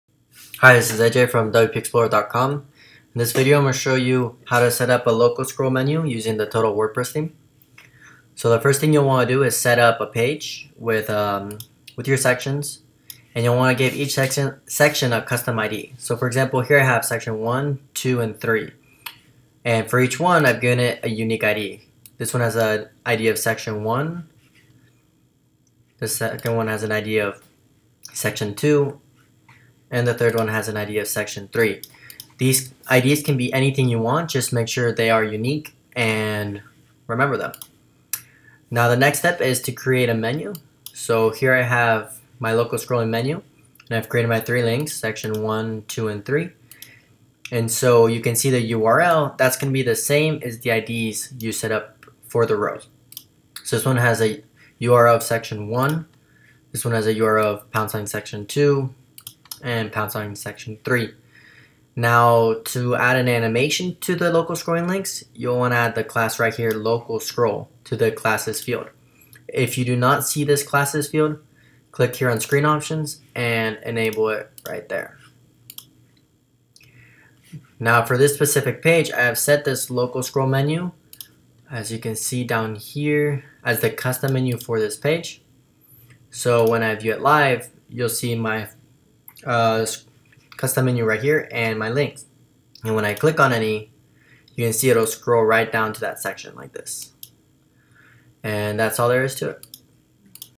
Local Scroll Menu - Total WordPress Theme
Quick video guide showing how to create a local scrolling single page menu in the Total WordPress theme.